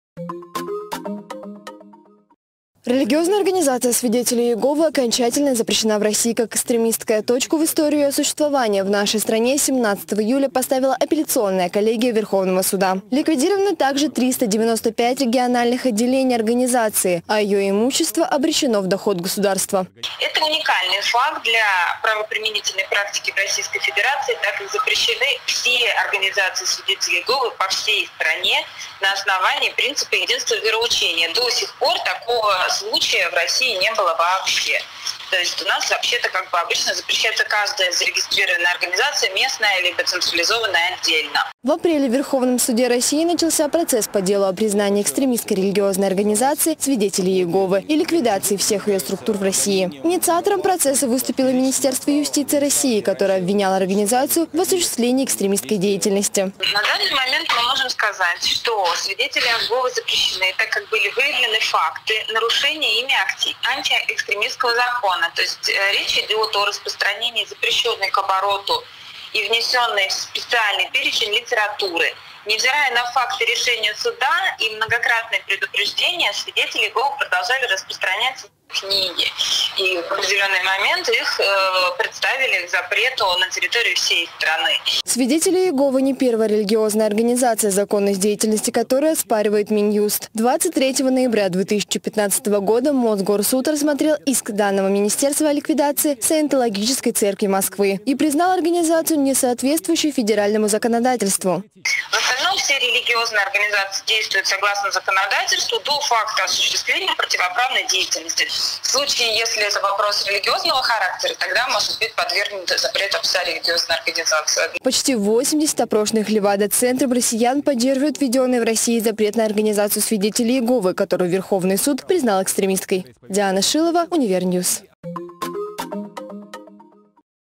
Свидетелей Иеговы «прикрыли»
17 июля Верховный суд РФ признал организацию «Свидетели Иеговы» экстремистской и распорядился конфисковать все ее имущество в пользу государства. О том, почему же это произошло и можно ли относить «Свидетелей Иеговы» к религиозным конфессиям, узнала Диана Шилова.